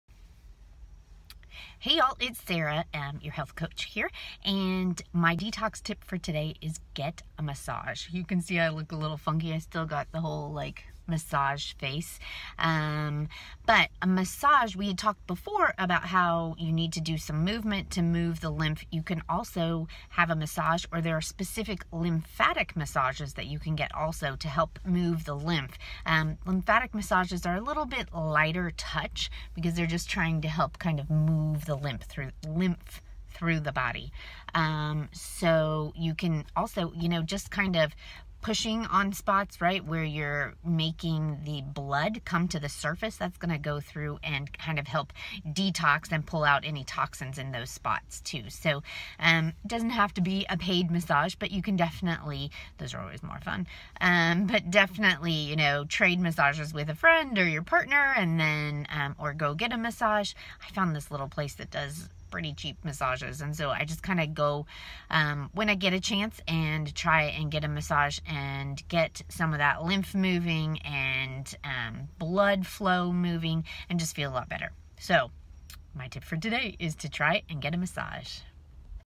Detox with a Massage - Your Health Coach Sarah
We’ve been talking about detoxing and today we add in massage. It’s not only for relaxation.